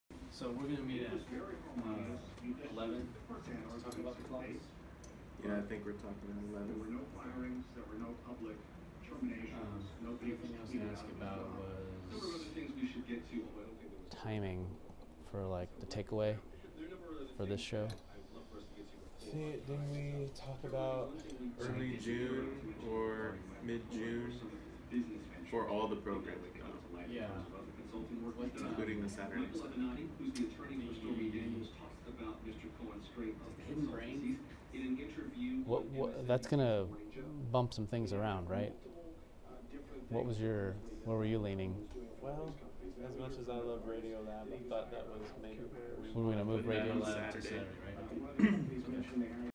Audio Test: Zoom F1 Lavalier vs iPhone
Wearing the Zoom F1 Lavalier mic while shooting with an iPhone. Text added to identify which audio source is present. Raw unprocessed audio.